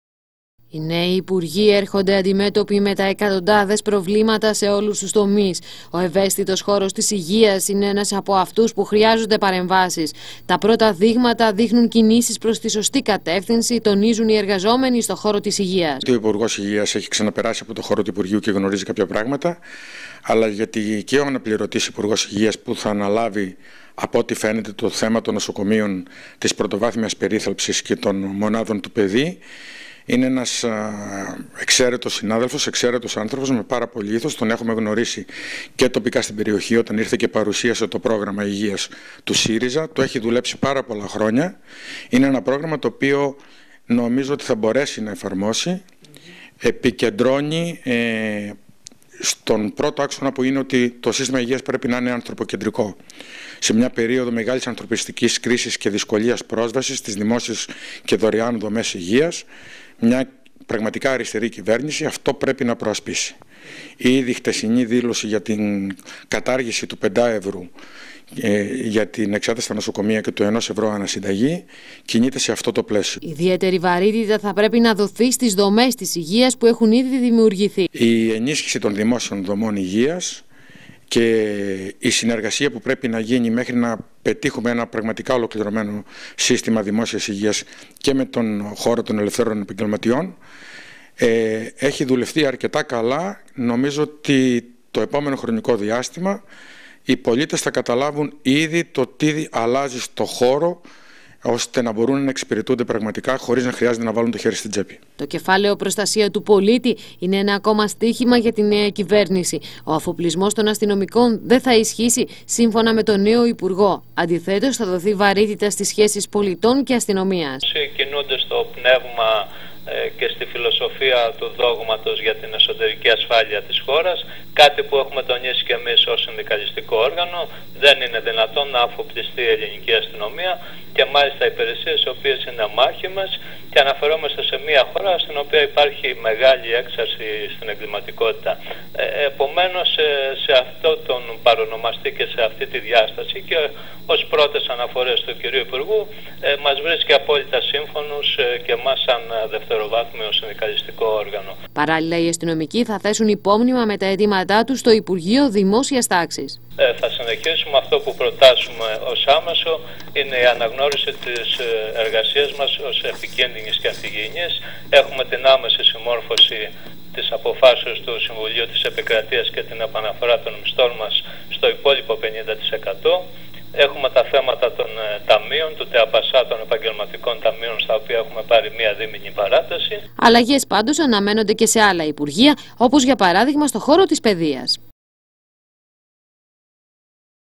ΜΕ ΟΡΕΞΗ ΓΙΑ ΔΟΥΛΕΙΑ ΟΙ ΝΕΟΙ ΥΠΟΥΡΓΟΙ
Οι νέοι  υπουργοί έπιασαν δουλειά από σήμερα και οι πολίτες περιμένουν λύσεις σε προβλήματα που χρονίζουν. Σε κάποιους τομείς έχει δοθεί η κατεύθυνση που θα ακολουθηθεί όπως για παράδειγμα στην αστυνομία όπου  διευκρινίστηκε ότι δεν θα αφοπλιστούν οι αστυνομικοί. Νευραλγικός τομέας είναι επίσης η υγεία όπου σύμφωνα με τους γιατρούς τα πρώτα δείγματα είναι θετικά.